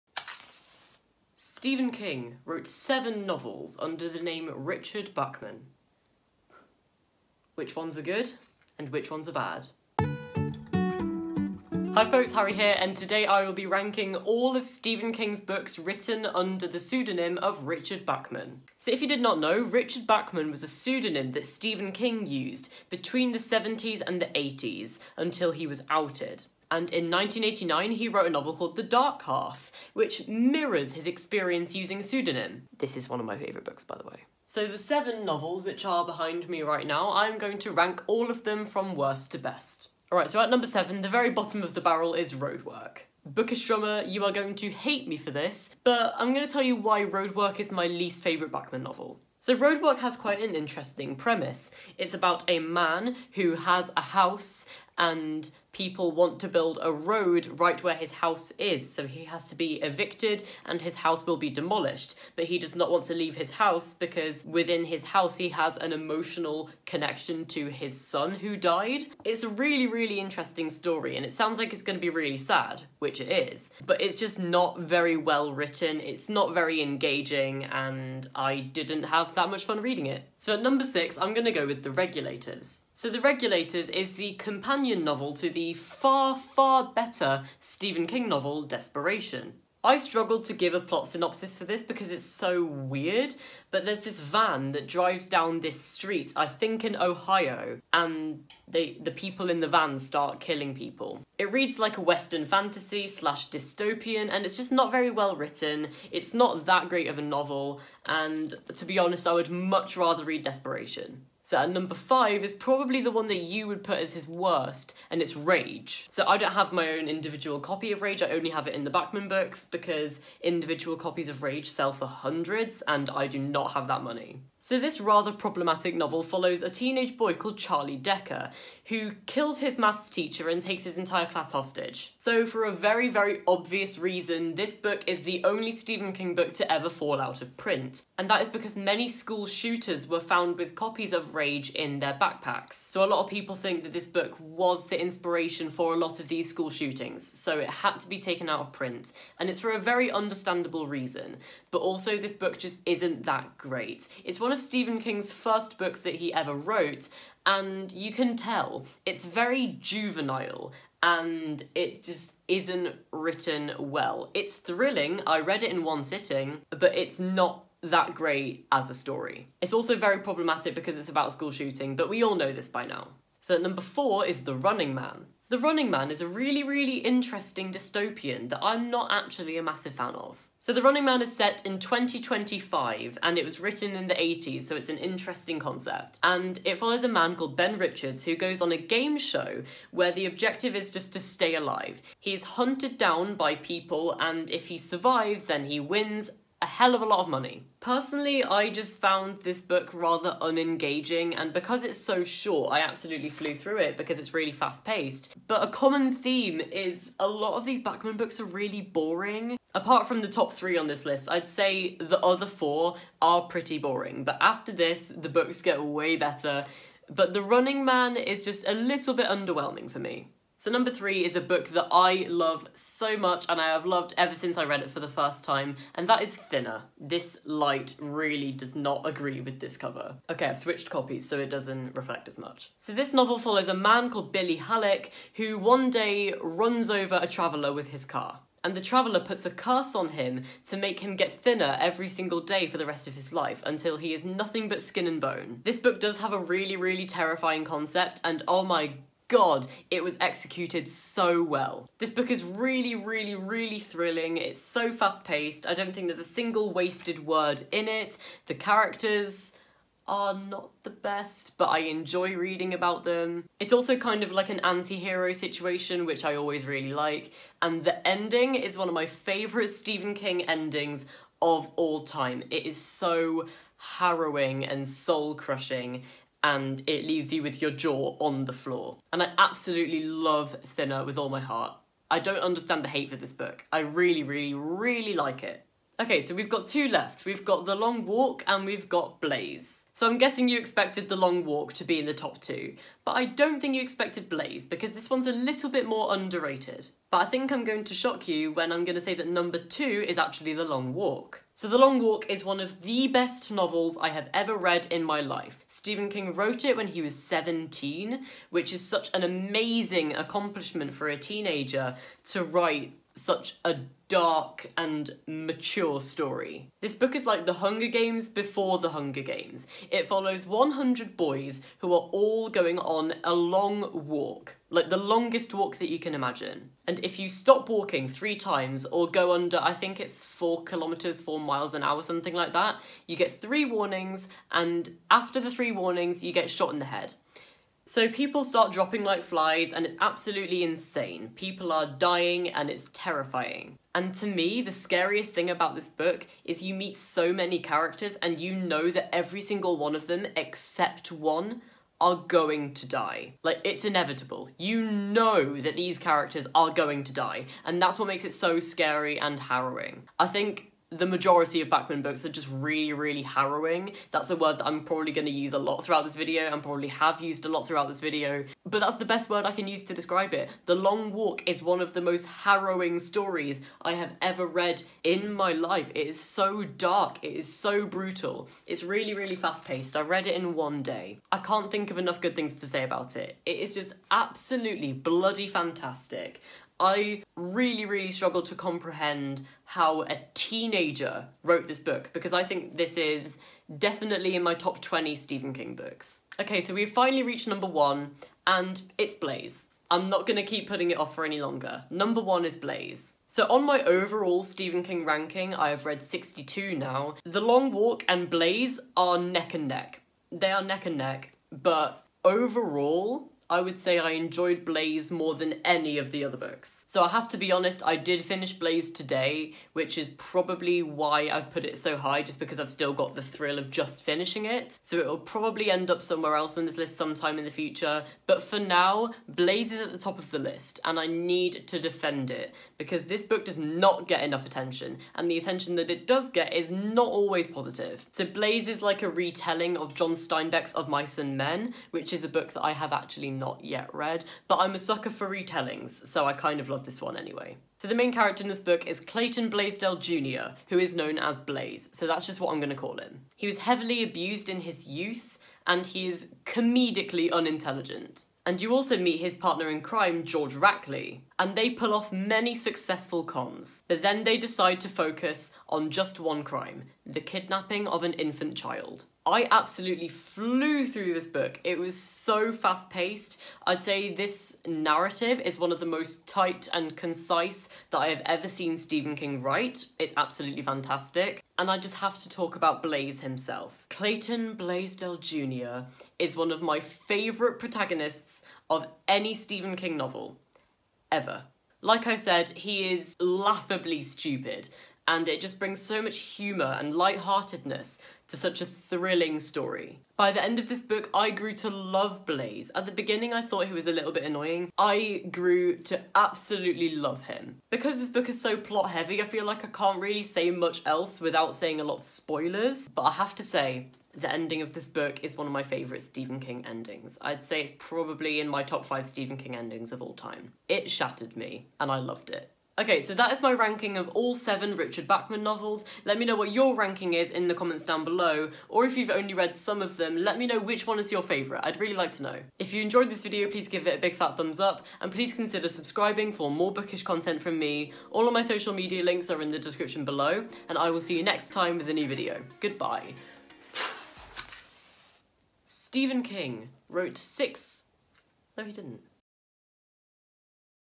EVERY RICHARD BACHMAN NOVEL RANKED! (Stephen King)
SOME OF THE BOOKS MENTIONED IN THIS VIDEO ARE NOT SUITABLE FOR SOME AUDIENCES.